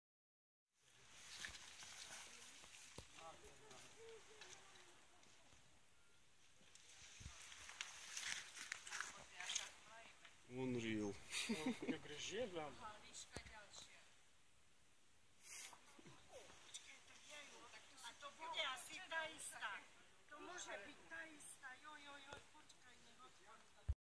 Crazy Fox
Wild fox in Slovakia High Tatras mountains not afraid to people